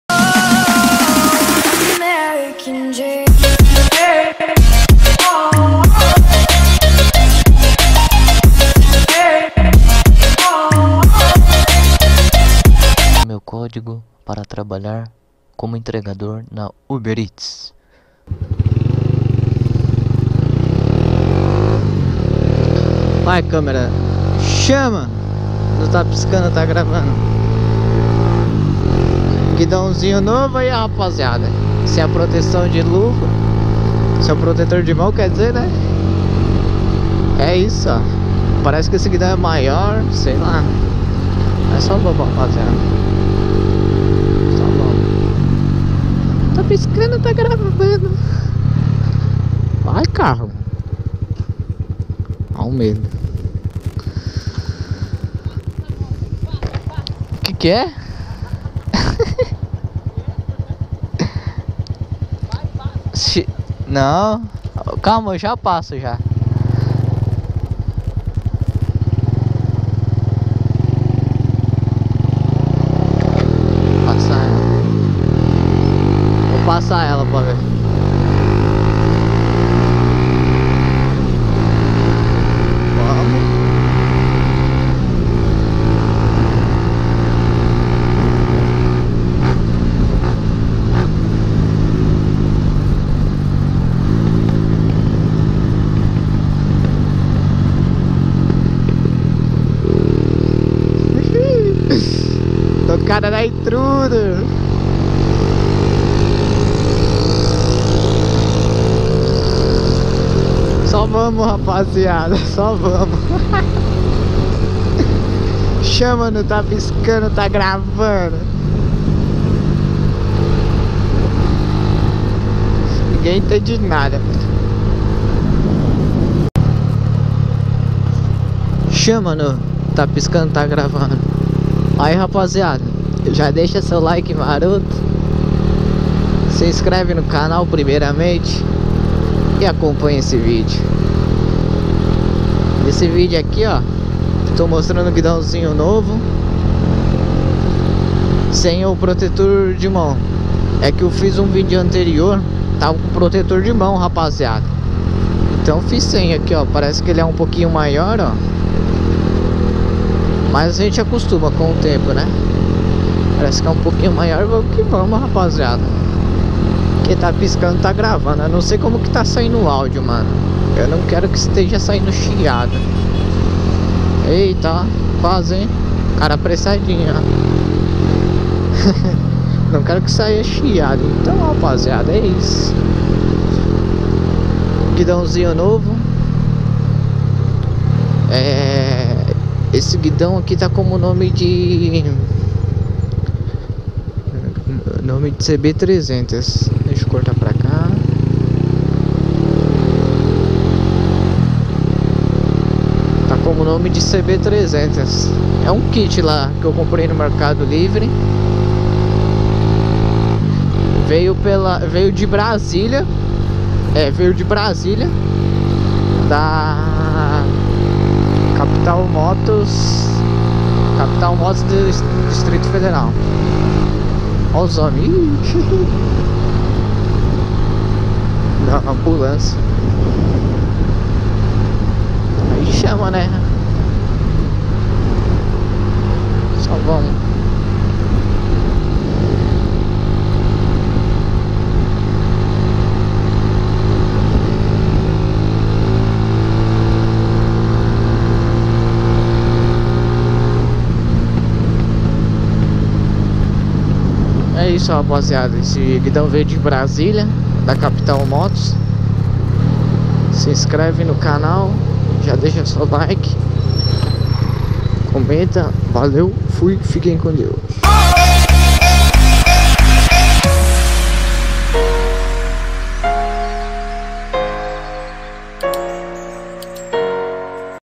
Guidão Novo na Intruder 125 !!
Bom Dia Boa Tarde e Boa Noite Nesse video mostro o guidao novo que coloquei na Intruder 125 é kit que comprei pelo Mercado livre vindo de Taguatinga Distrito Federal Brasília é um guidao esportivo CB 300 junto com manoplas na cor do guidao assim muito bom de usar e vale a pena comprar e recomendo para você que quer da uma modificada na moto e espero que gostem do video deixa seu like se inscreve no canal e compartilha .